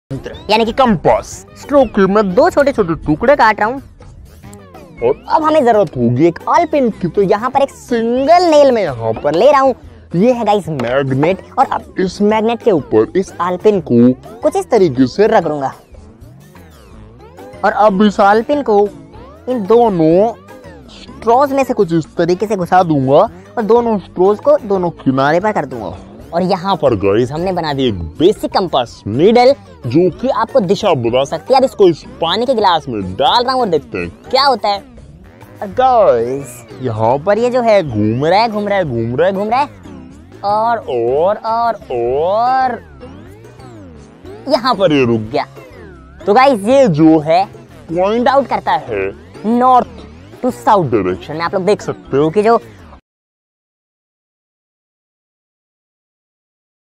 My first video🥰 | how to make compass at home😜✌️
A small magnet (such as a refrigerator magnet)
A needle (such as a sewing needle)
A cork or piece of foam (to float the needle)
A plastics material or basin filled with water
Instructions:

Take the needle and stroke one end of it repeatedly in one direction along the magnet. This magnetizes the needle.
Take the cork or foam and make a small indentation in the center, just large enough for the needle to rest in.
Place the magnetized needle in the indentation of the cork or foam so that it can float freely.
Place the cork with the needle in the bowl or basin filled with water.
Once the cork and needle have come to a stop, the needle will align itself in a north-south direction (the north end pointing towards the Earth's magnetic north pole).